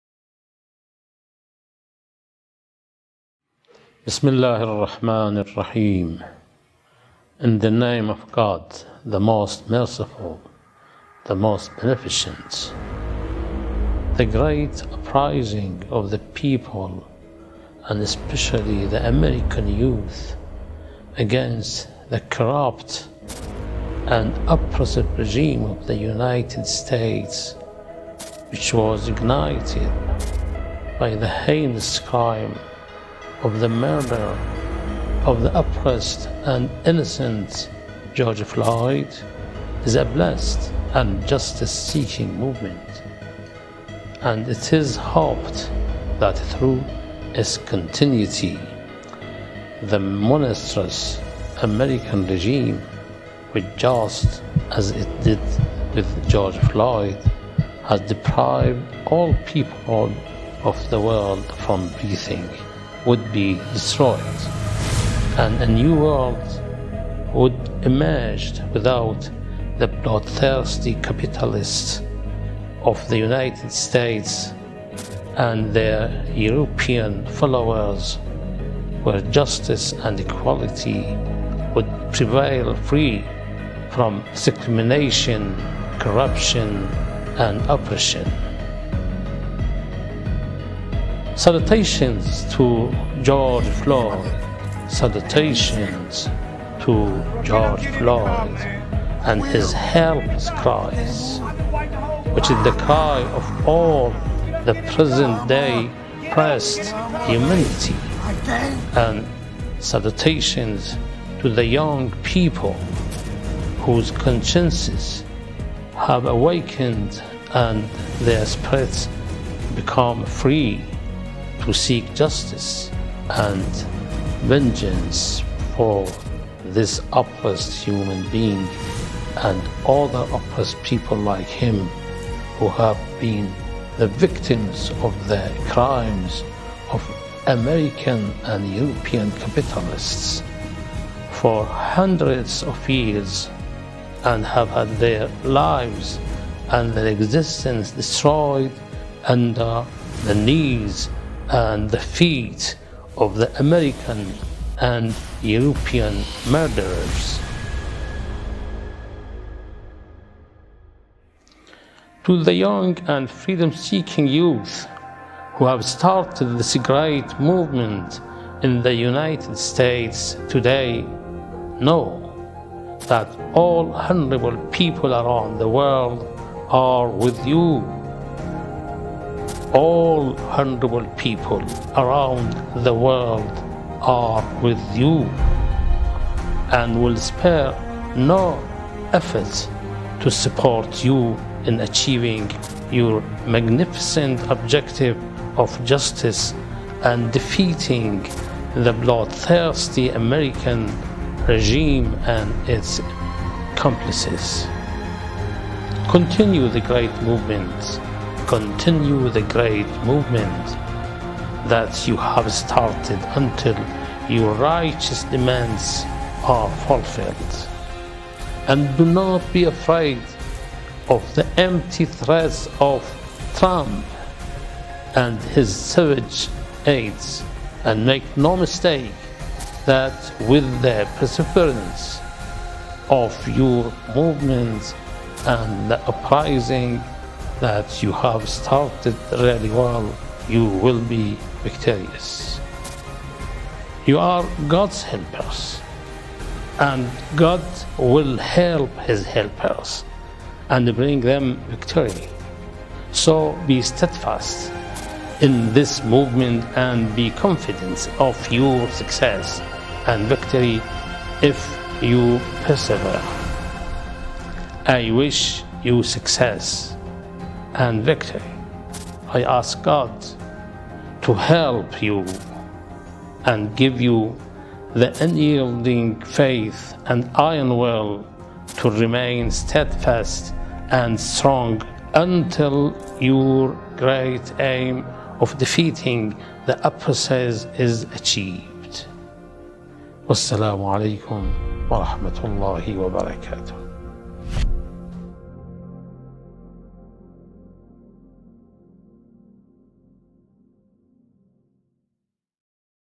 The protests in USA - Ayatullah Mohsen Araki
Ayatullah Mohsen Araki talks about the protests in USA, George Floyd and describes the protests as blessed and the protesters as God's helpers.